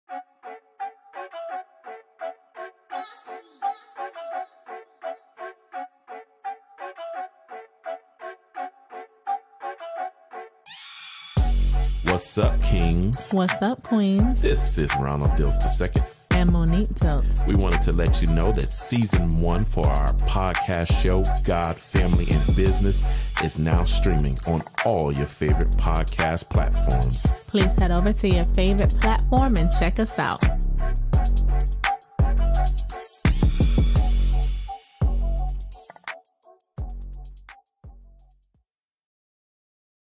GOD, FAMILY, & BUSINESS PODCAST - SEASON 1 (TEASER)
Ronald Diltz II & Monique Diltz reflect on how three simple principles keep them grounded: GOD, FAMILY, & BUSINESS.